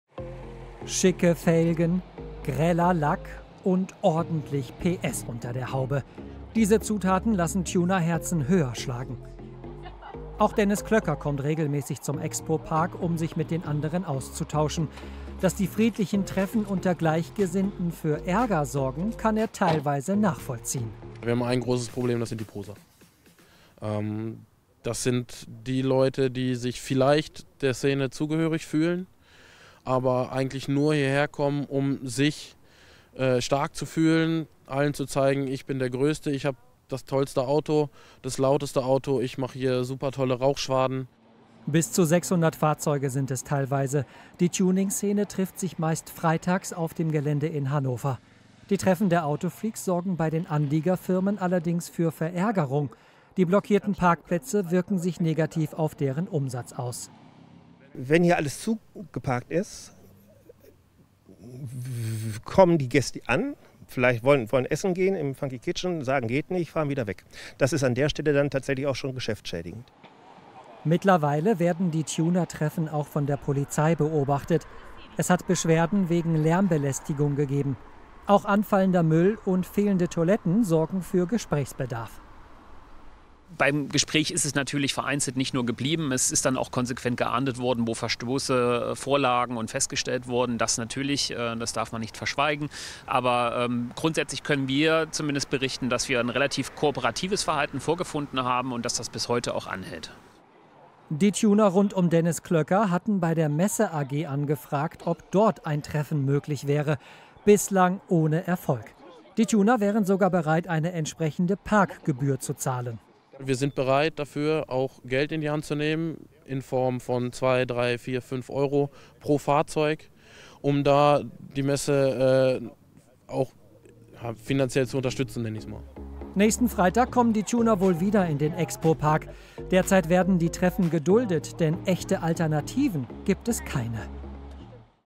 Tuner-Treffen am Expo-Park in Hannover sorgt für Ärger
In Hannover (Niedersachsen) sorgt bei einigen die Tuningszene für Ärger. Fahrer:innen treffen sich mit ihren Autos meist freitags im Expo Park. Dabei kommen auch schon mal 600 aufgemotzte Fahrzeuge zusammen. Das stört die umliegenden Unternehmen, denn Lärm und blockierte Parkplätze sind schlecht für deren Geschäft.